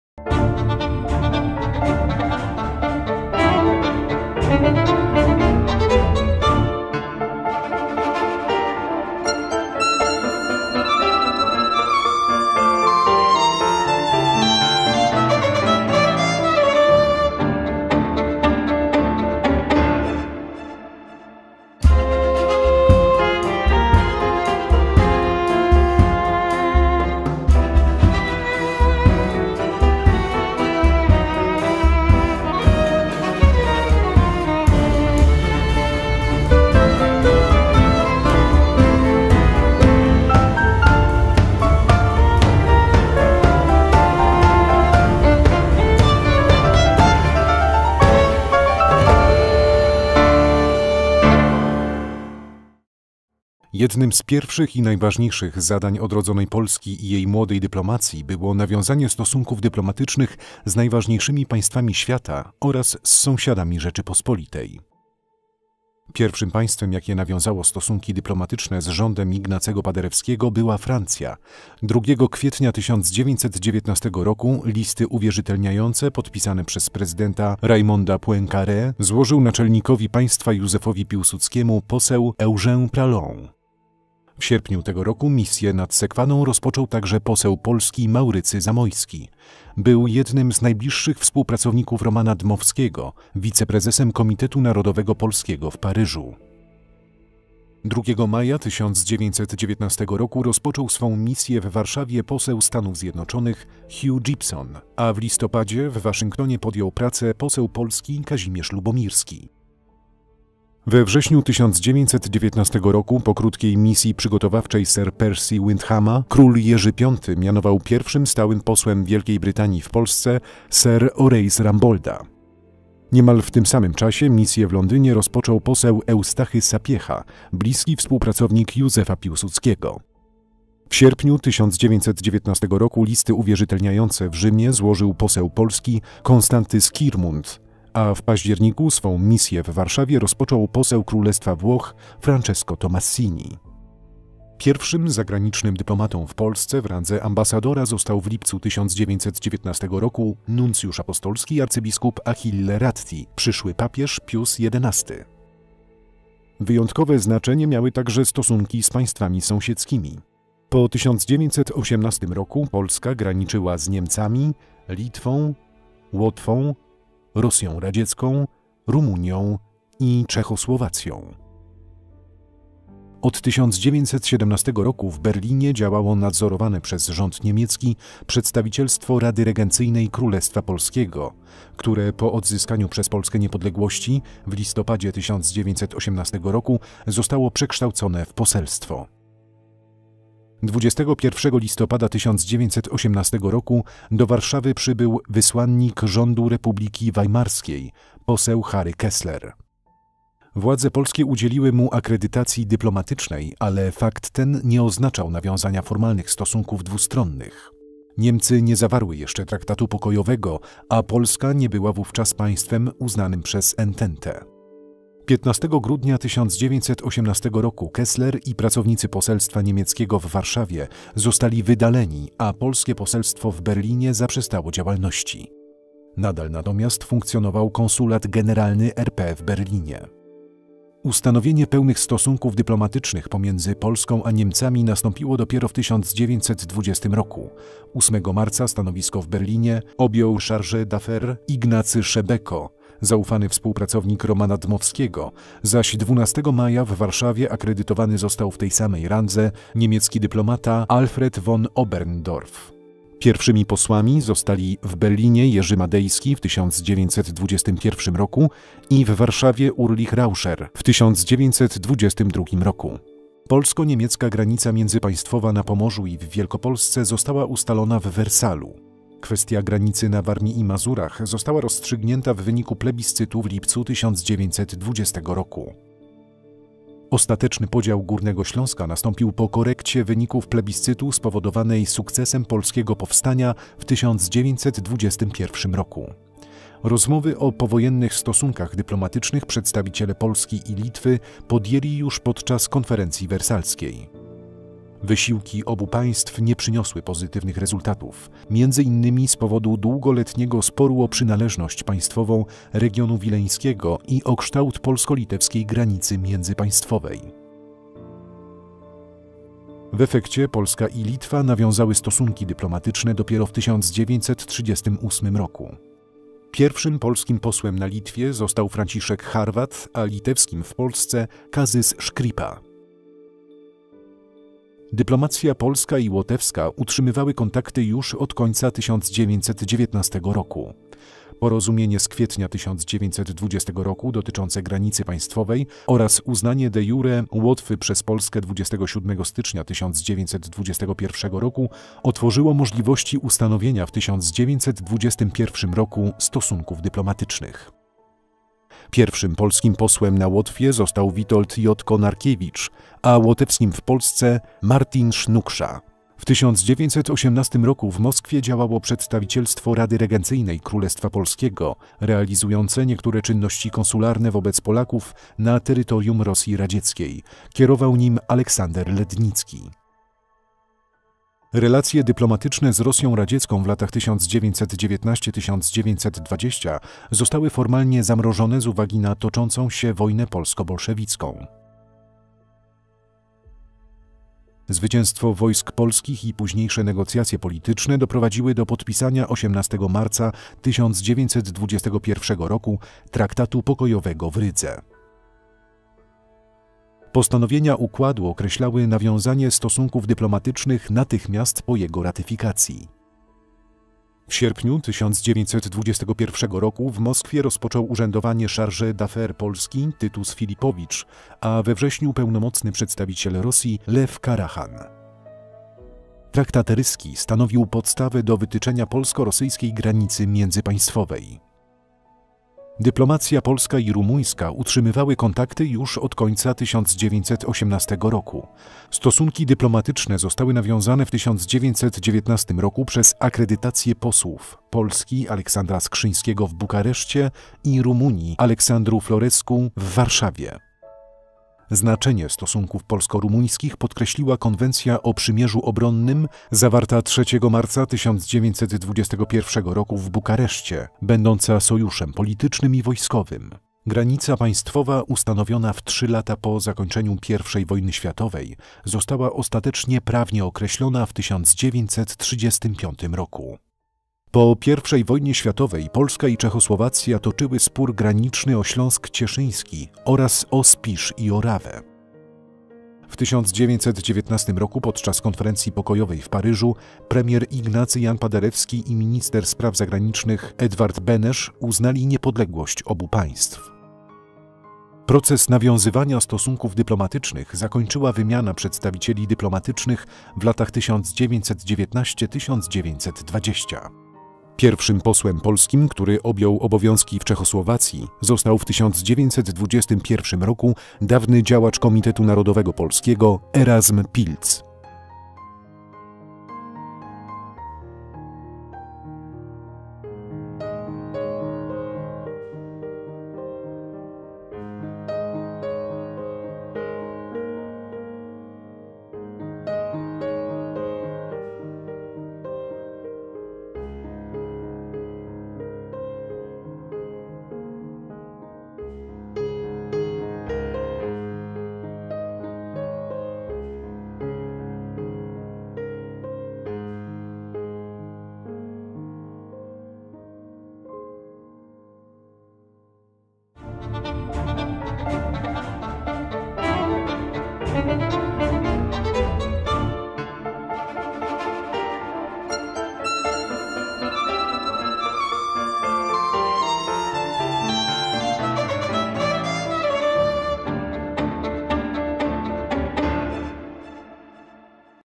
Odcinek 3 Nawiązywanie stosunków dyplomatycznych
Odcinek 3 cyklu filmów o początkach polskiej dyplomacji w 1918 roku przygotowany przez Ministerstwo Spraw Zagranicznych z okazji Dnia Służby Zagranicznego obchodzonego 16 listopada 2020 po raz dwunasty. W tym roku, ze względu na pandemię COVID-19 zrezygnowano z tradycyjnych wydarzeń towarzyszących temu świętu. Wychodząc naprzeciw zaistniałej sytuacji MSZ przygotował specjalną zakładkę na stronie która poświęcona została Dniowi Służby Zagranicznej oraz cykl 4 krótkich filmów dokumentalnych. Filmy dostępne są również
Zapraszamy.